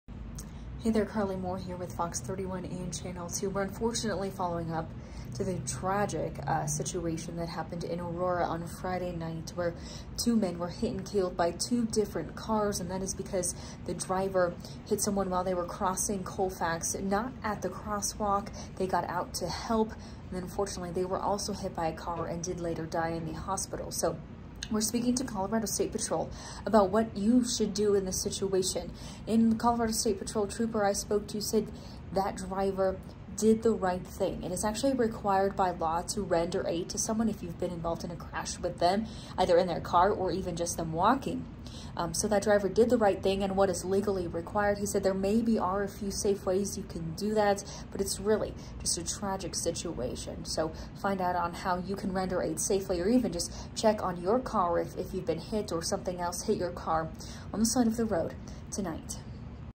Driver did right thing in helping injured man after crash
Two men were struck and killed by different vehicles Friday night. Colorado State Patrol offers advice on what drivers can do if they are involved in an emergency situation like that.